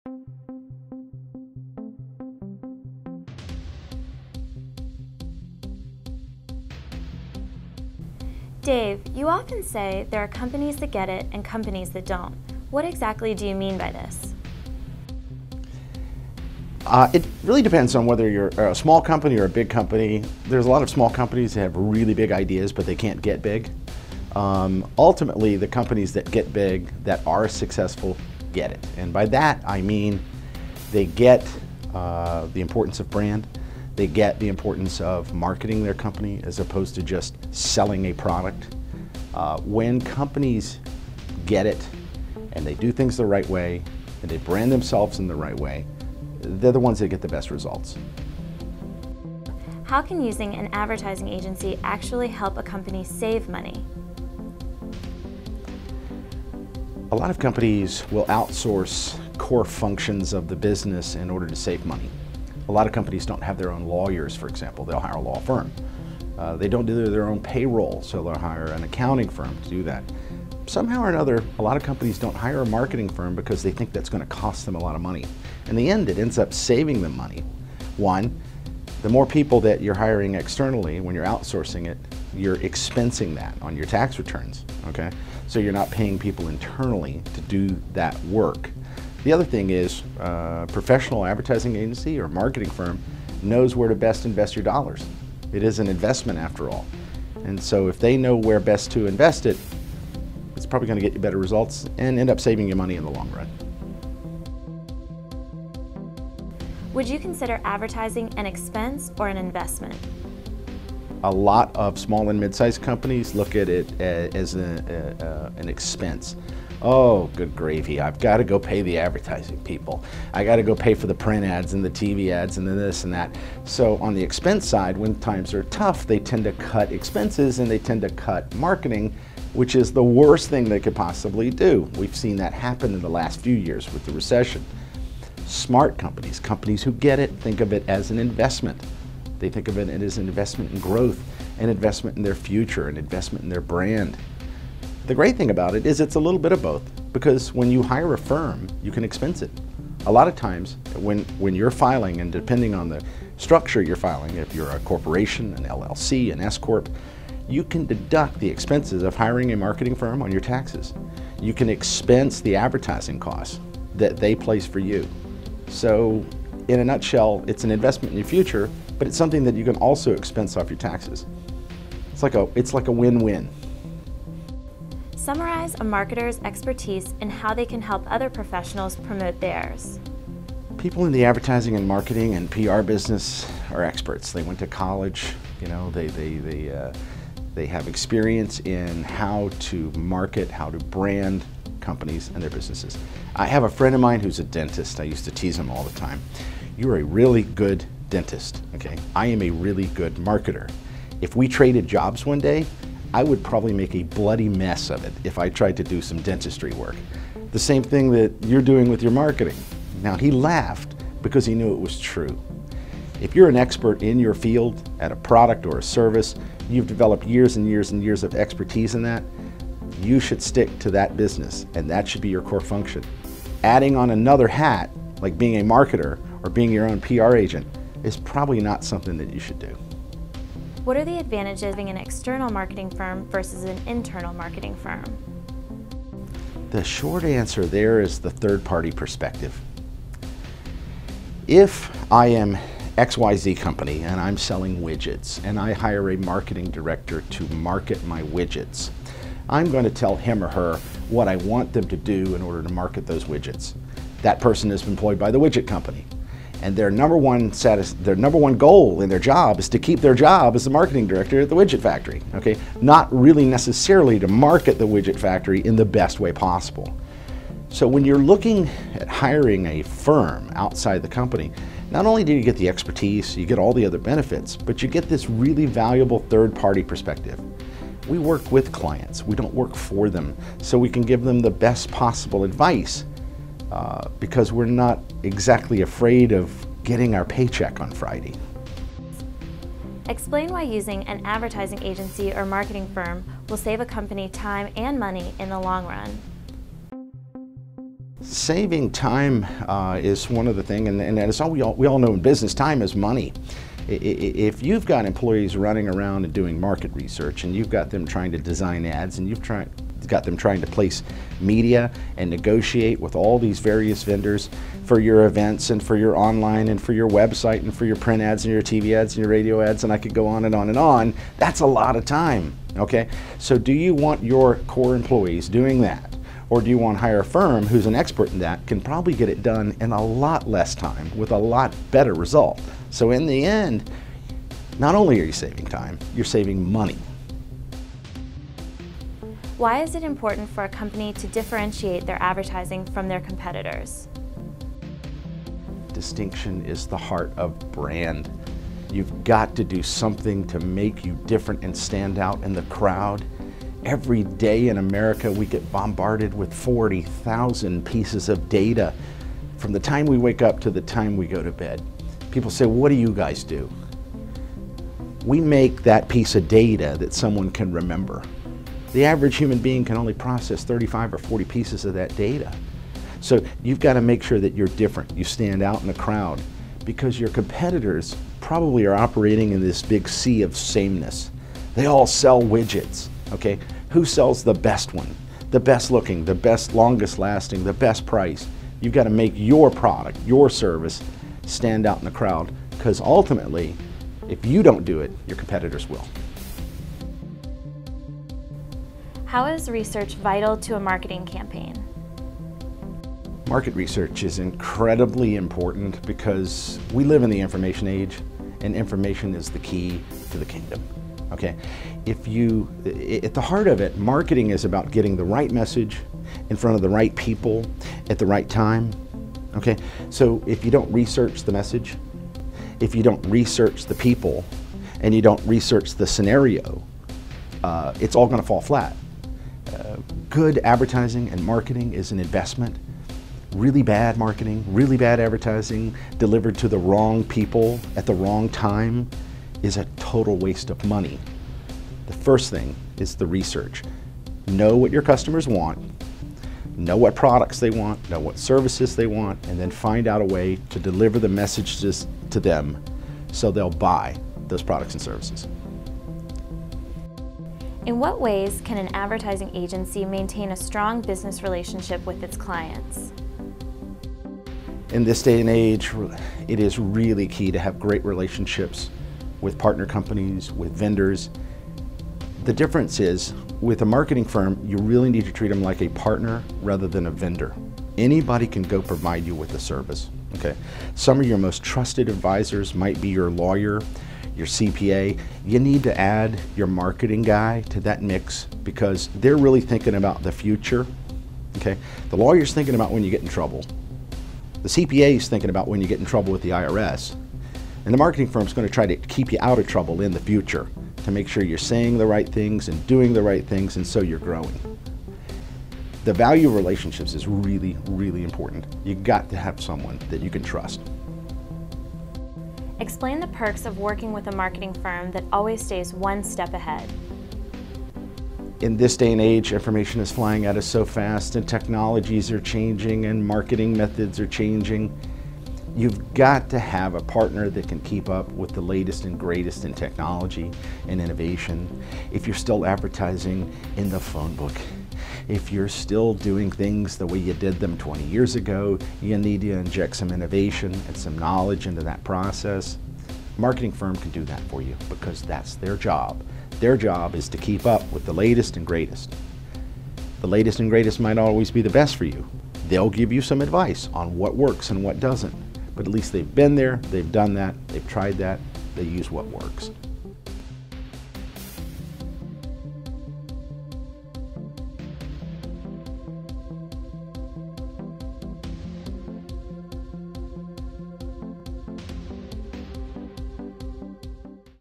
Top 10 Reasons to Hire a Marketing Firm
Thinking about hiring an ad agency or marketing firm? Interested in boosting sales? Or are you simply looking for an innovative way to build your brand? If so, check out Madison+Main's Chief Idea Officer, Dave Saunders, talking about the top ten reasons why your company should hire a marketing firm.